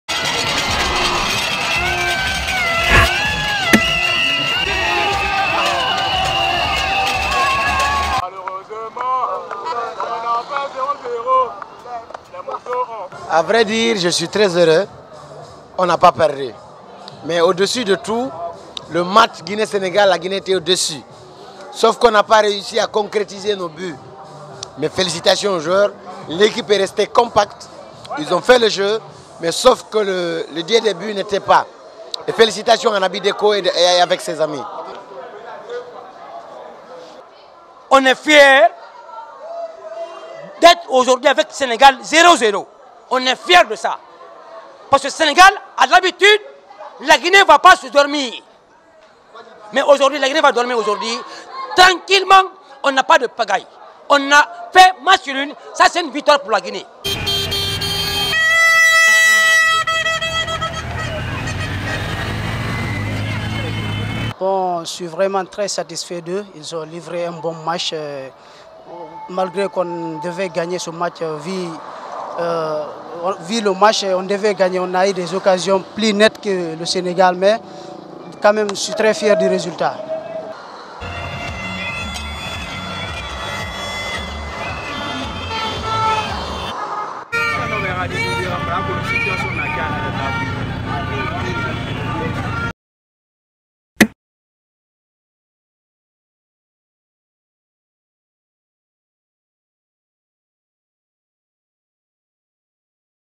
CAN au Cameroun: Guinée–Sénégal, un nul à la saveur d'une victoire pour les Guinéens
A Conakry, ce vendredi soir, le match nul obtenu entre la Guinée et le Sénégal dans le cadre de la deuxième journée des matchs de poules du groupe B de la CAN 2021 à fait beaucoup d'heureux.